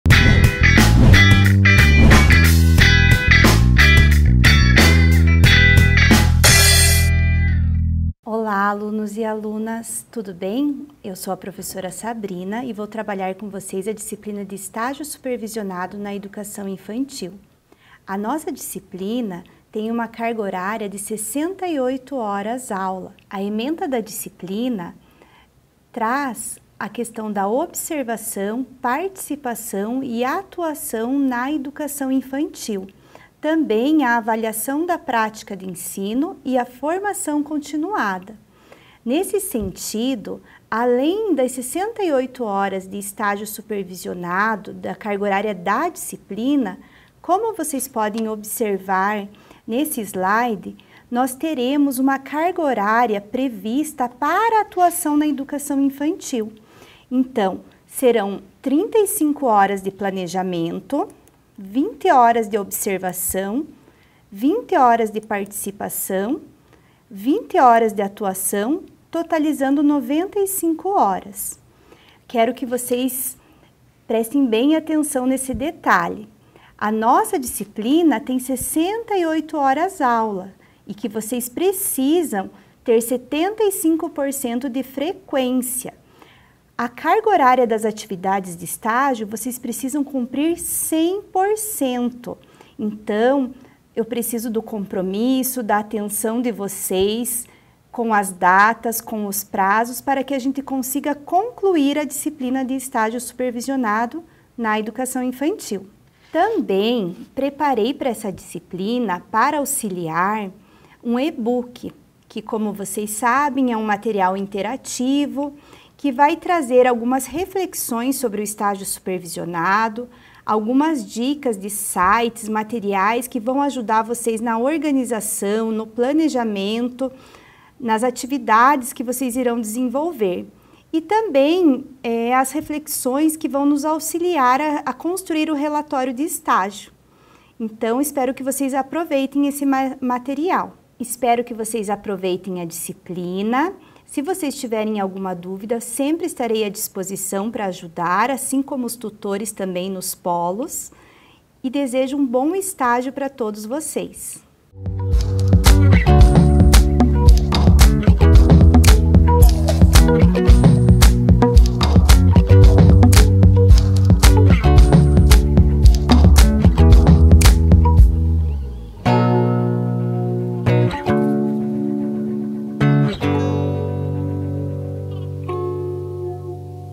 [PED] - Estágio Supervisionado na Educação Infantil - Apresentação
Apresentação da disciplina de Estágio Supervisionado na Educação Infantil com a professora Sabrina Plá Sandini  para o curso de Pedagogia - EaD da Universidade Estadual do Centro Oeste do Paraná.